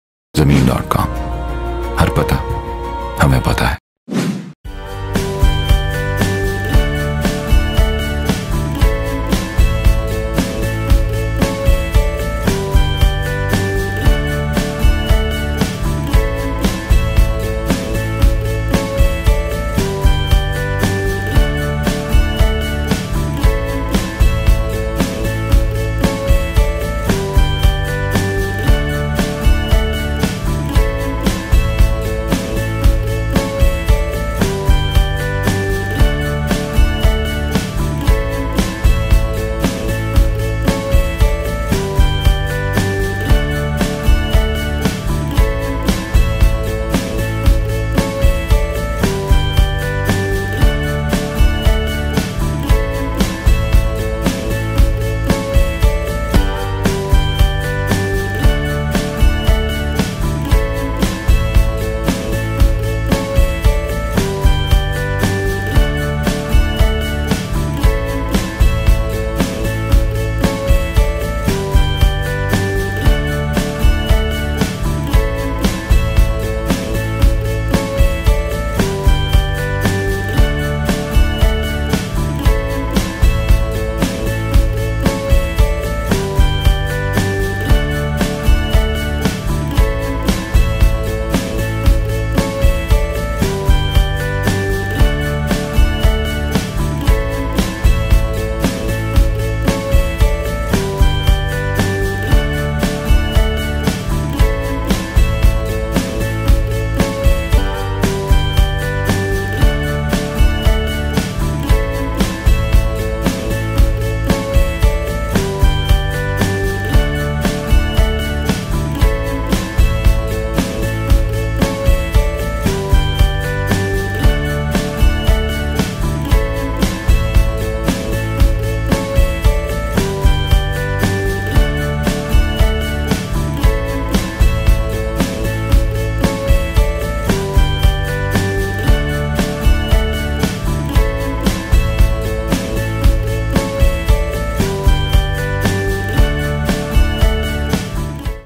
2 KANAL RESIDENTIAL PLOT FOR SALE IN SECTOR M-2 LAKE CITY LAHORE
We are pleased to offer a beautiful Residential Plot for sale at a very reasonable market price of Rs 29,000,000.
1 Kanal Pair Plot.
It is sure to be a great opportunity to invest your hard earned money as ongoing infrastructure development makes it ideal for all kind of investment purposes. The preeminent Residential Plot is situated on an ideal location and has got an easy approach to nearby area as well.